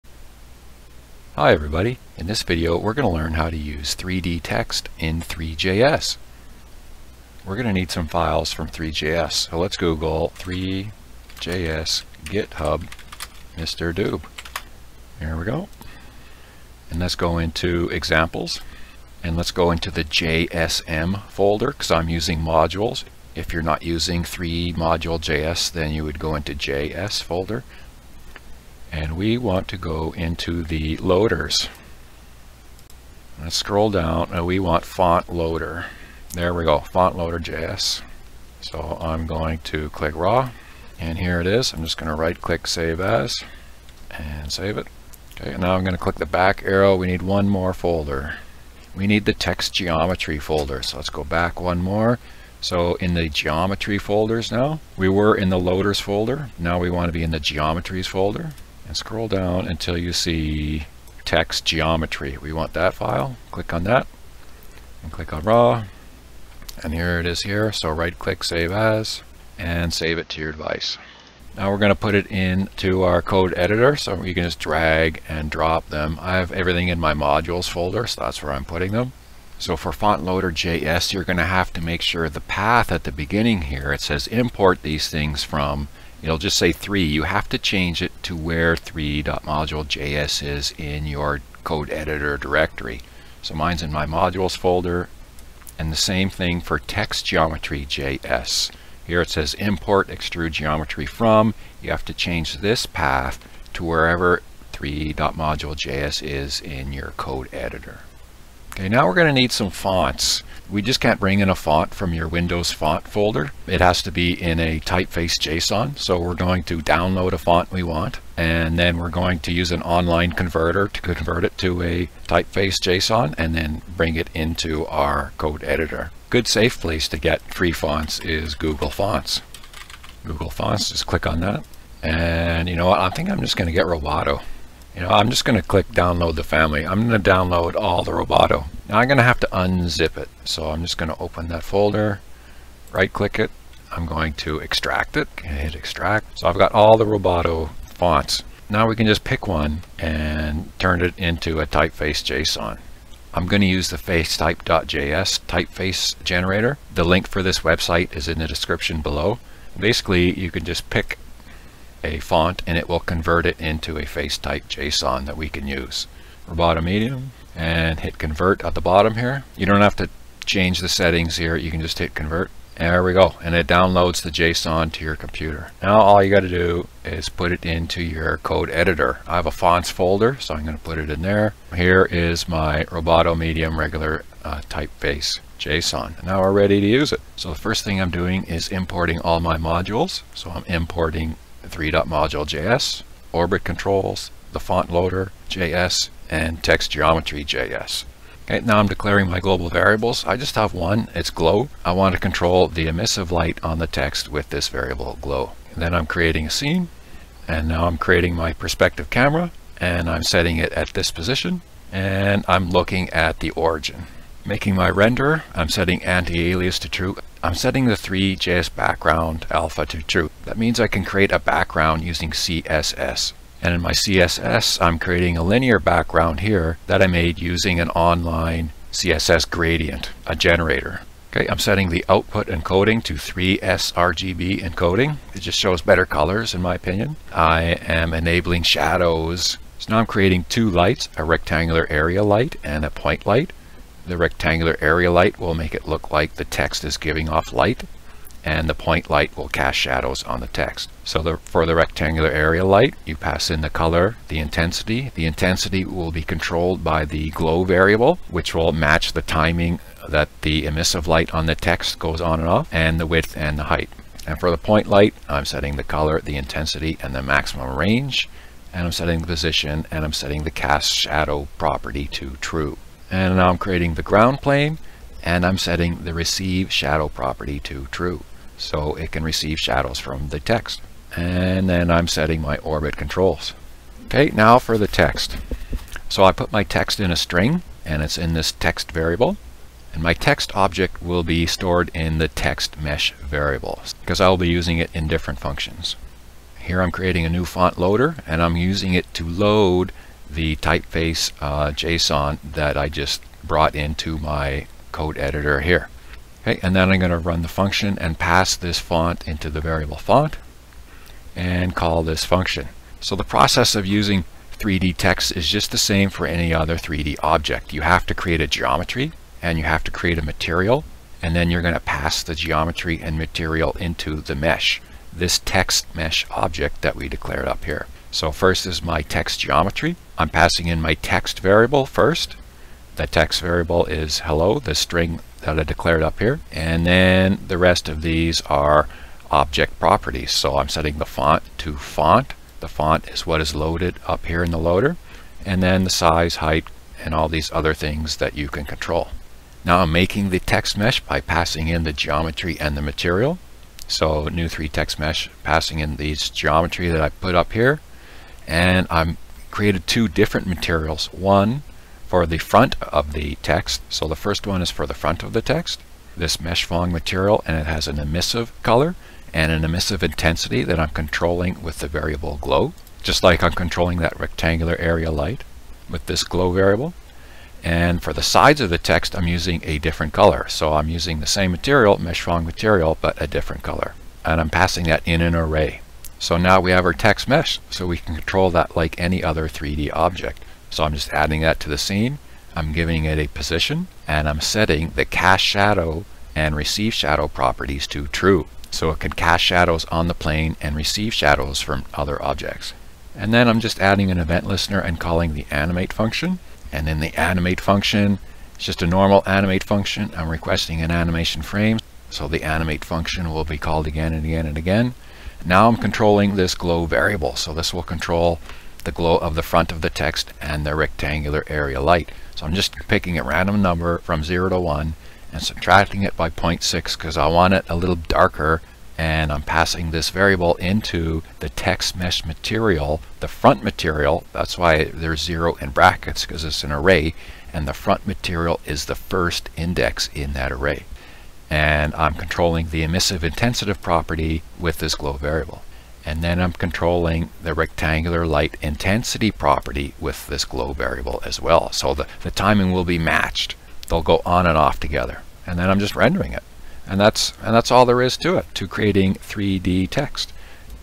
17a How to make 3d text in three.js
Learn how to convert font to TypeFace JSON.
Learn how to create 3D Text.
Learn how to turn on shadows for renderer and objects.
Learn how to control emissive light and RectAreaLight to simulate reflection of emissive light.

Timestamps:
00:00 Intro/demo
00:07 How to get FontLoader.js & TextGeometry.js
01:48 How to get free fonts
02:29 How to convert font to typeface JSON
03:07 Import statements for modules
03:18 Declare global variable
03:25 Declare scene and camera
03:34 Renderer: how to set alpha background to make CSS background
03:59 How to make RectAreaLight and PointLight
04:33 Make ground plane to receive shadows
04:41Orbit controls
04:43 How to make 3D text three.js
06:43 Animate function to control text emissive light and RectAreaLight

Resources:
Three.js TextGeometry Doc: